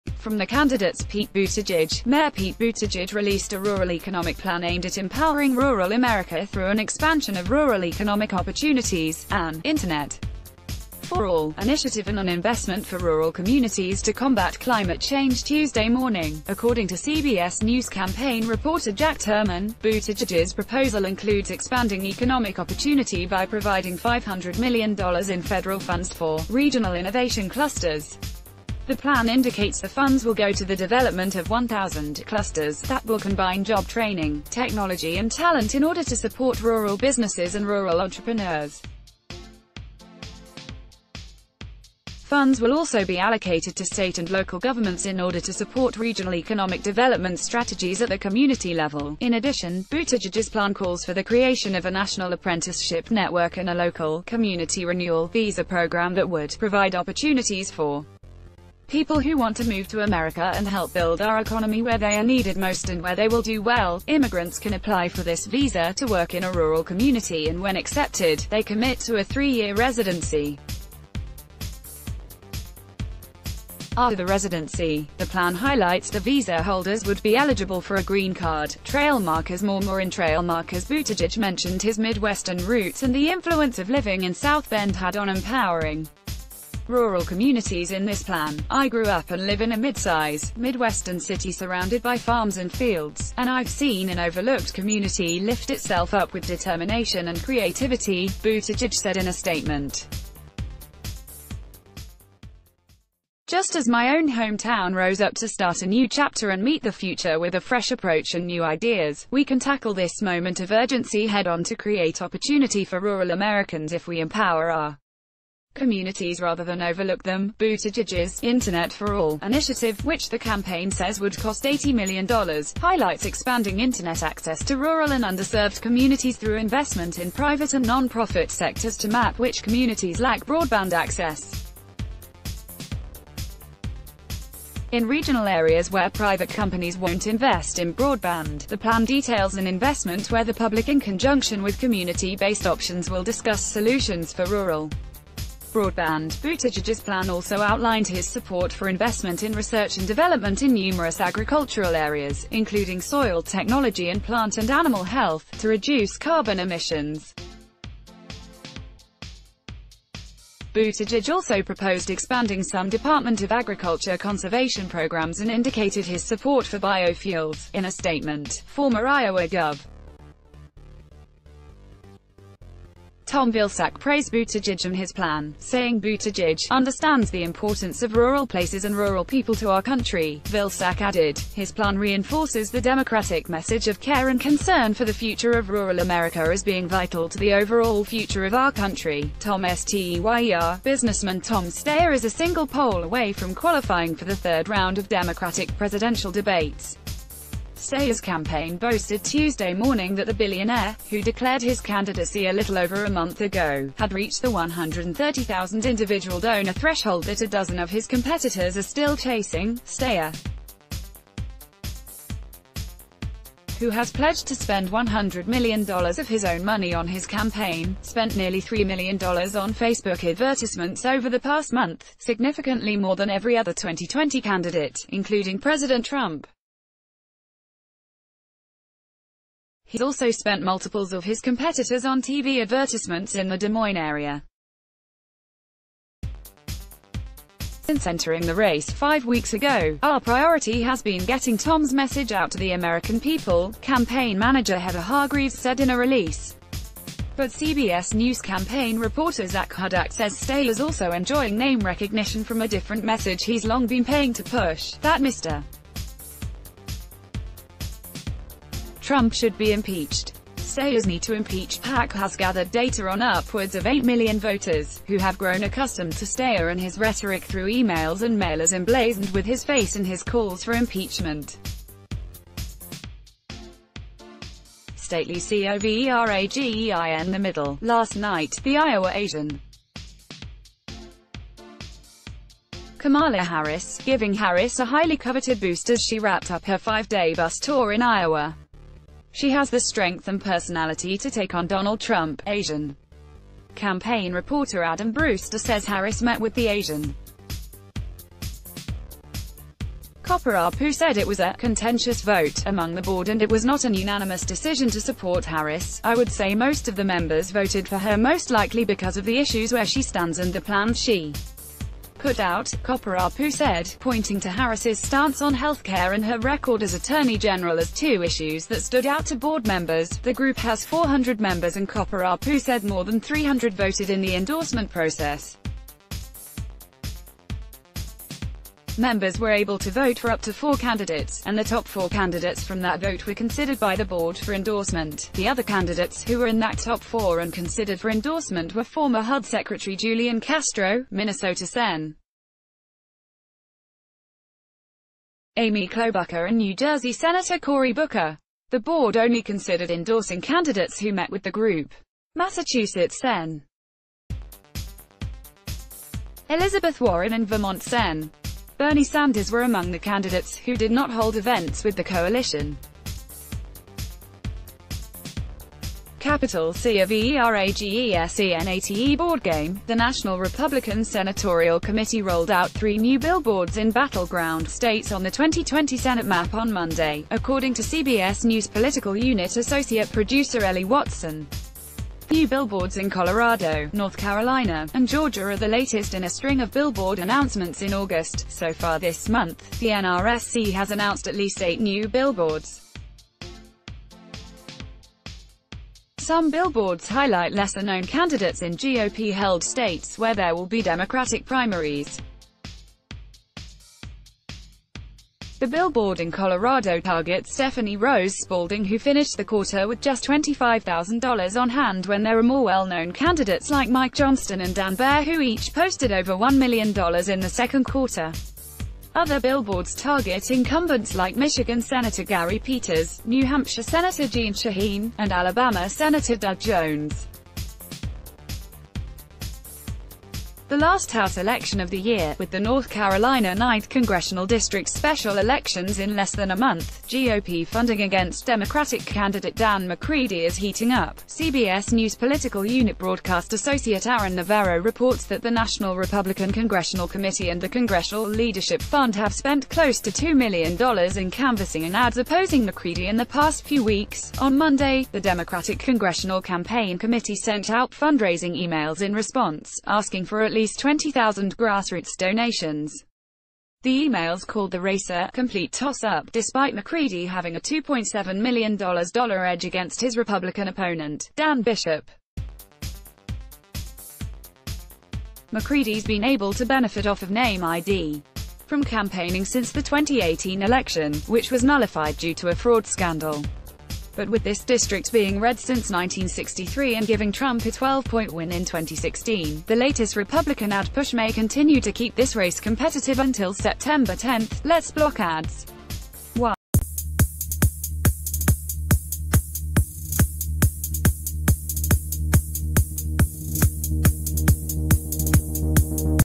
| Breaking News USA2020 Daily Trail Markers: Inside Buttigieg's "Internet for All" initiative
FROM THE CANDIDATES                                                                                          PETE BUTTIGIEG: Mayor Pete Buttigieg rele...